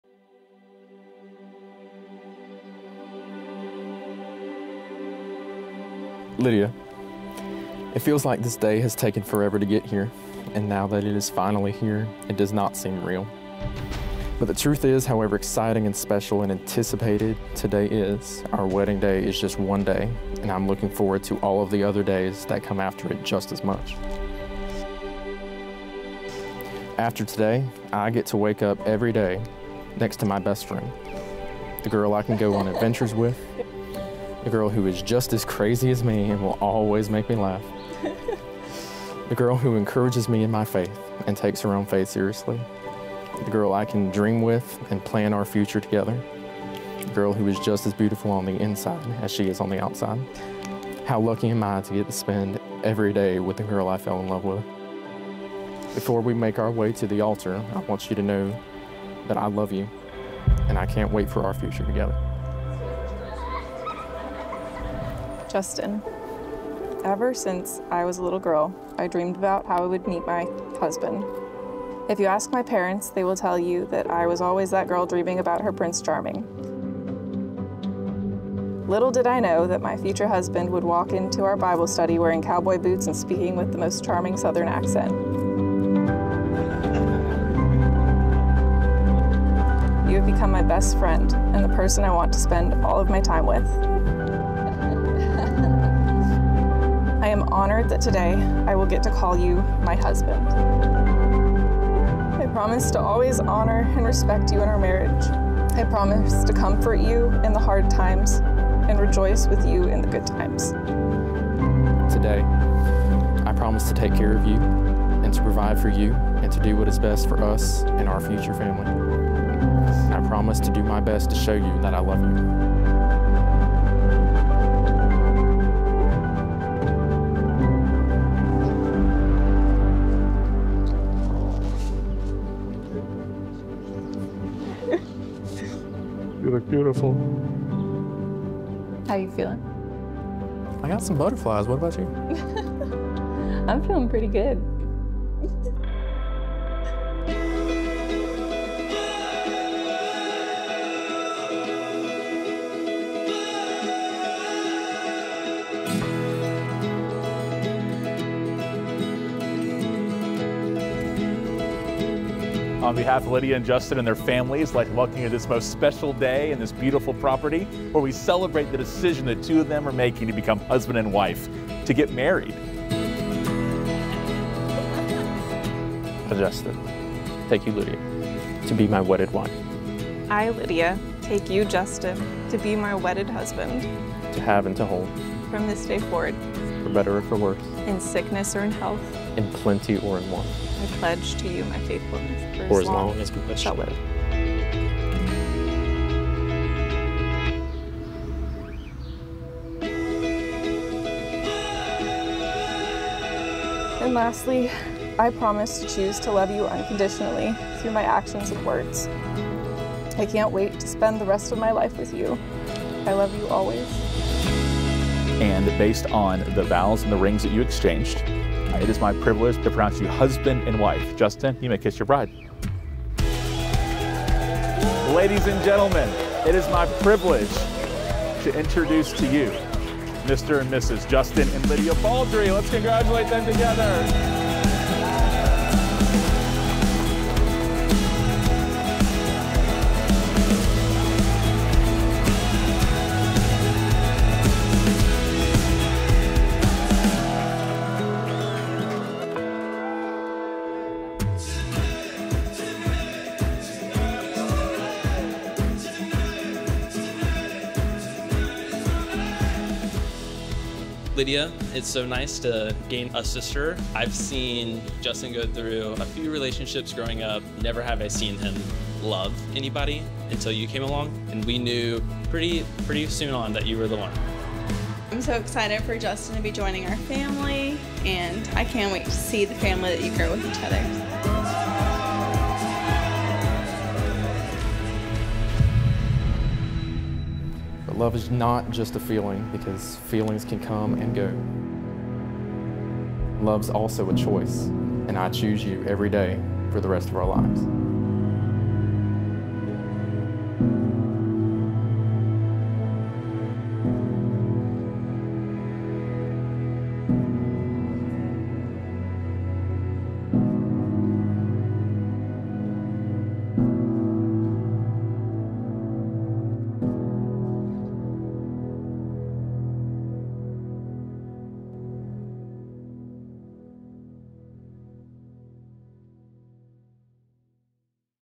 "I Choose You Everyday" Bride & Groom's Emotional Wedding Vows / Wedding Film BMPCC6KPro + Sony a7IV
This beautiful couple, Justin and Lydia, exchanged emotional wedding vows alone before their ceremony and we were there to capture it on film. Their charming wedding celebration in South Georgia included exchanging emotional wedding vows pre-ceremony, a first look with the father of the bride, toasts and an intimate private last dance in their reception venue.
Shot on Blackmagic Pocket Cinema Camera 6K Pro, Blackmagic Pocket Cinema Camera 6K, and the Sony a7IV.